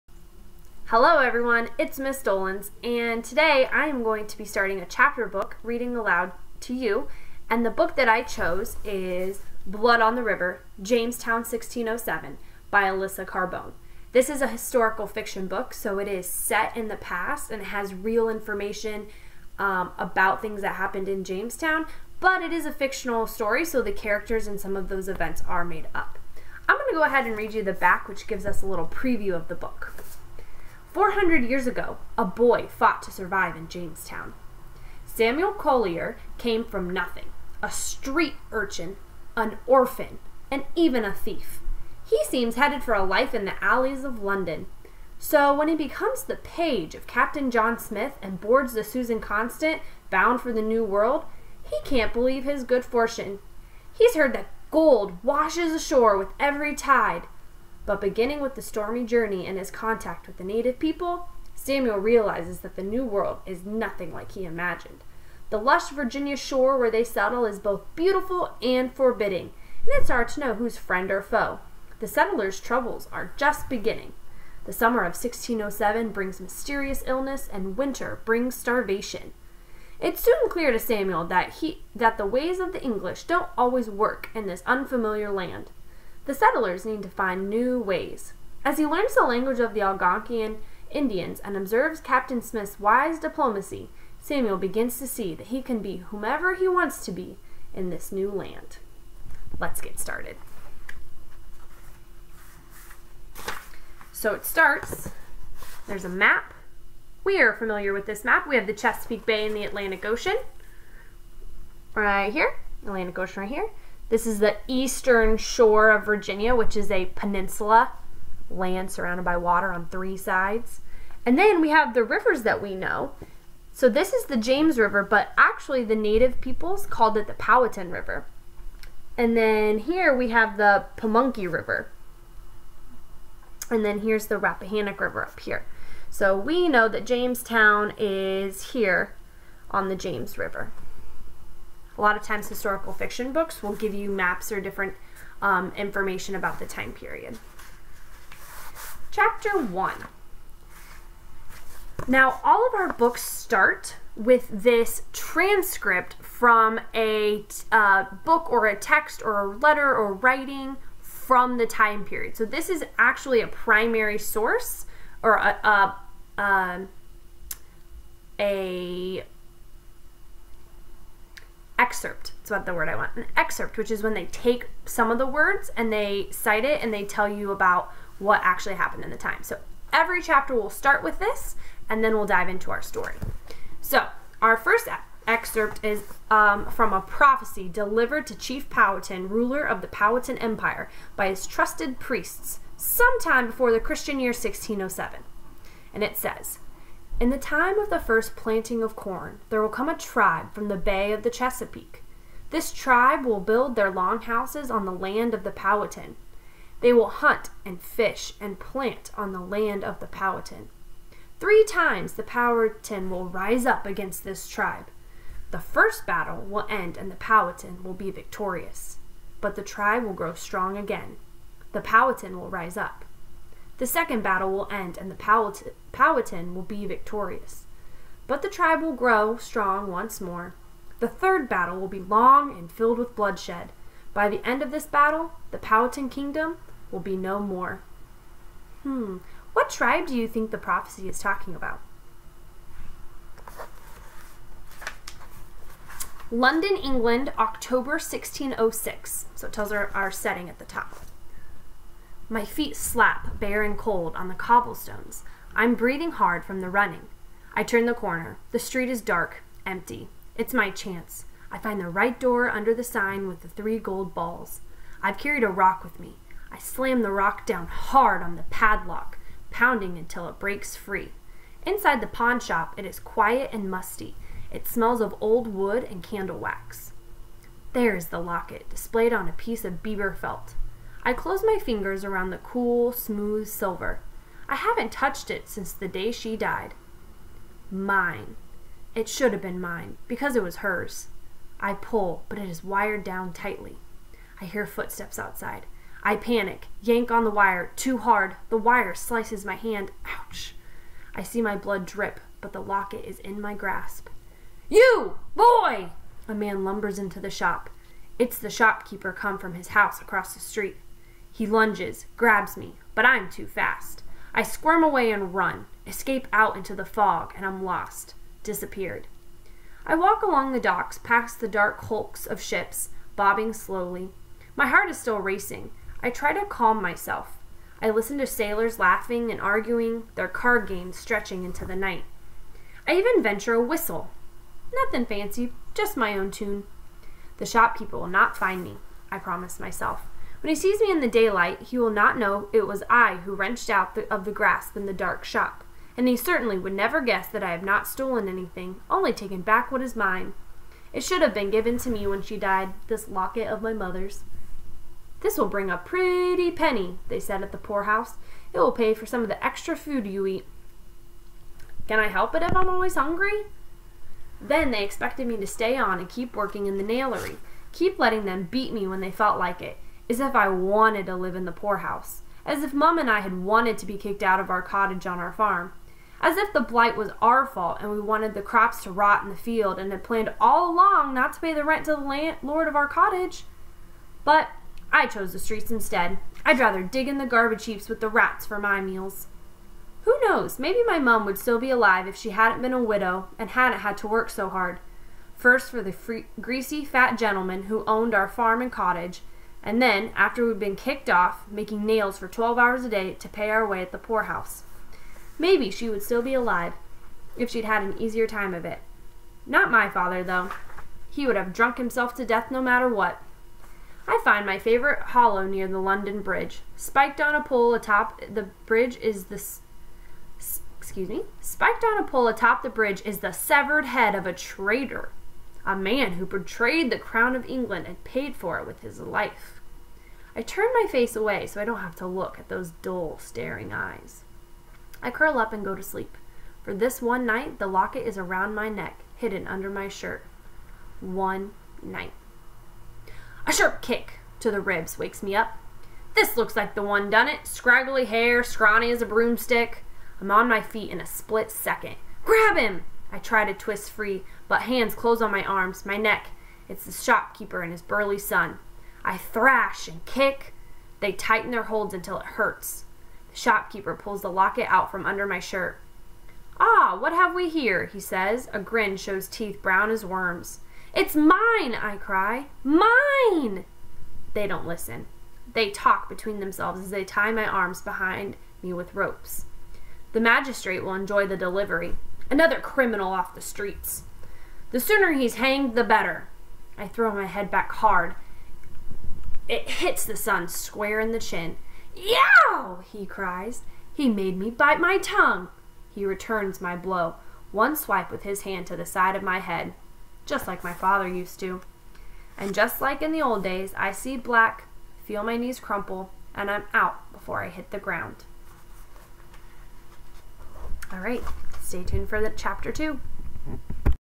Blood on the River: Intro & Chp 1
Ms. Dolenz is introducing Blood on the River by Elisa Carbone and reading chapter 1.